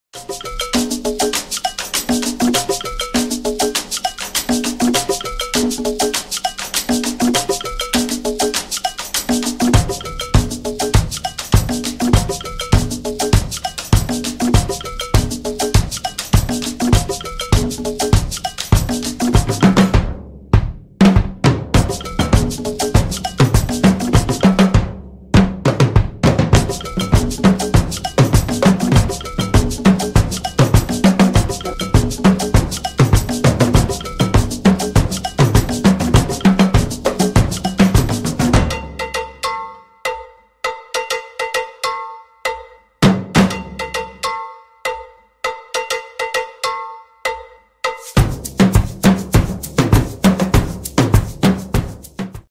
Body Painting Art festival
body painting festival at kolkata ICCR. West Bengal India. it's a amazing festival and also eye catching and beautiful Art festival. bodypainting crazy short.